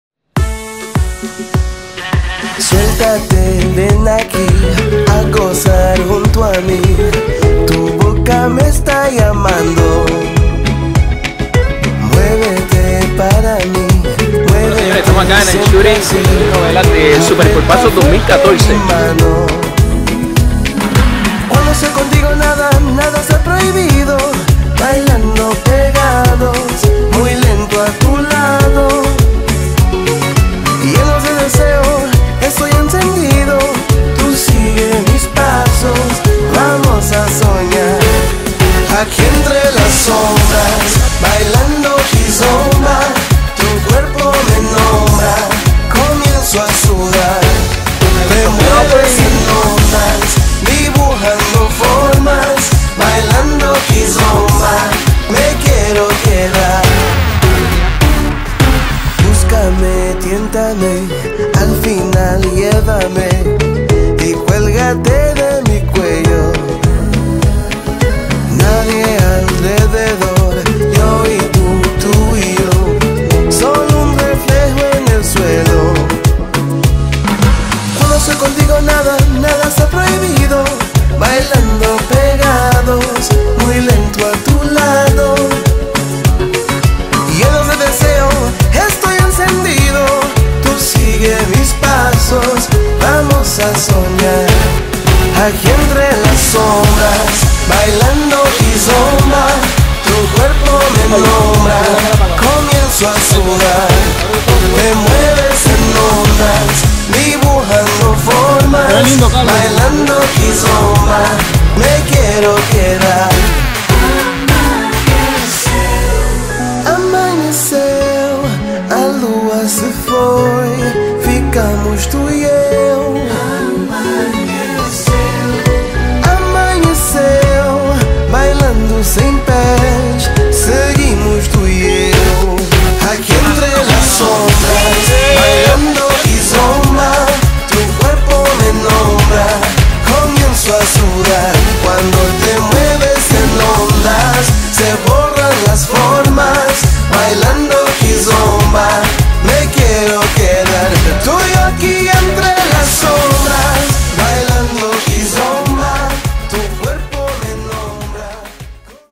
Cuerpazos 2014 Making Of
Making of del "Shooting" de  Cuerpazos Edición 2014

Llegó el verano y TVyNovelas P.R. te presenta la edición  de cuerpazos bajo el concepto del mundial de fútbol. Disfruta, como lo hicimos nosotros, un shooting de un grupo espectacular.

Jaime Mayol
Jose Figueroa
María del Pilar Rivera
Melwin Cedeño
Mimi Pabón
Osvaldo Friger
Saritza Alvarado

Director editorial
   William M. Venegas
Fotógrafo
   Dino Velvet
Making of / Video
  Lio Gómez
Asistente de fotógrafia
  Delio César Durán
Reporteros
  Neisha Molina
  Omar Matos
Maquillaje
  Jesús Cancel
  Reynaldo Maldonado
Locación
  Hotel Intercontinental / San Juan
Música Cortesía de:
  Coreón Dú
  Andrés Levin Publishing - Peer Music